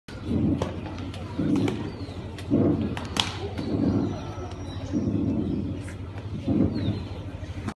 SHICS - VUNAPOPE EASTER PASSION PLAY 2022_stay tune for the full video.
Every year Sacred Heart International Catholic School-Vunapope host a Para Liturgy Easter Passion Play where the students portray the events of Easter. This year was no exception.